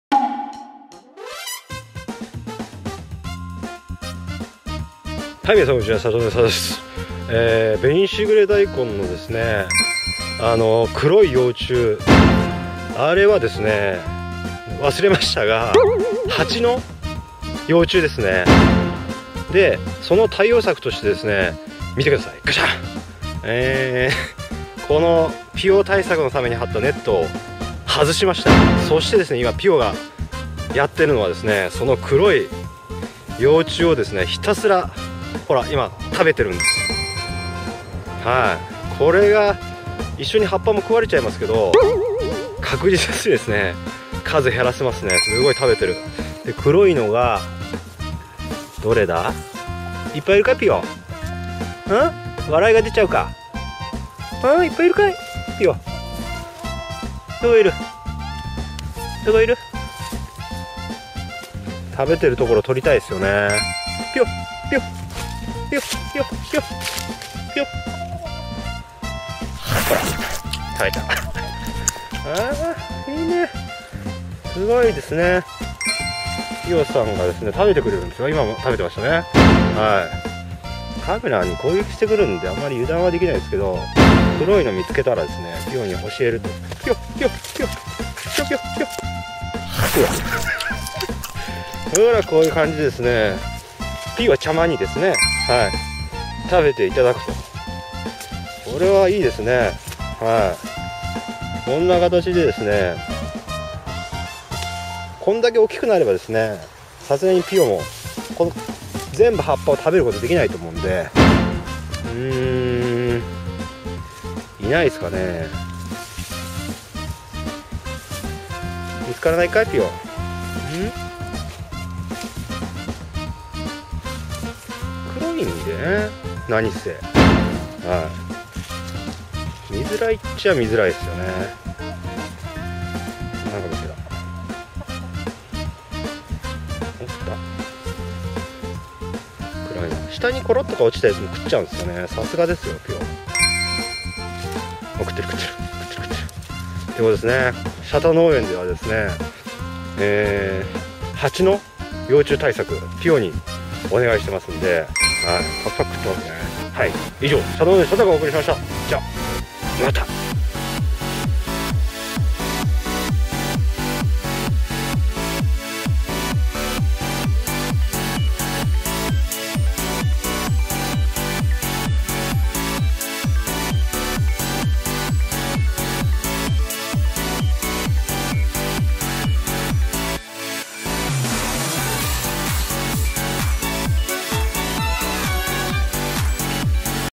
危機迫る！紅しぐれ大根！救うのはぴよだ！／Approaching crisis ! Success of the chichen " Piyo" .／きゅうり農家／きゅうり栽培／愉快なshata農園
ども！シャタ農園のシャタです！
農園に大量発生するカブラハバチ！これは便利で安全な方法！
鶏ぴよ様様です！

Approaching crisis ! Success of the chicken " Piyo" .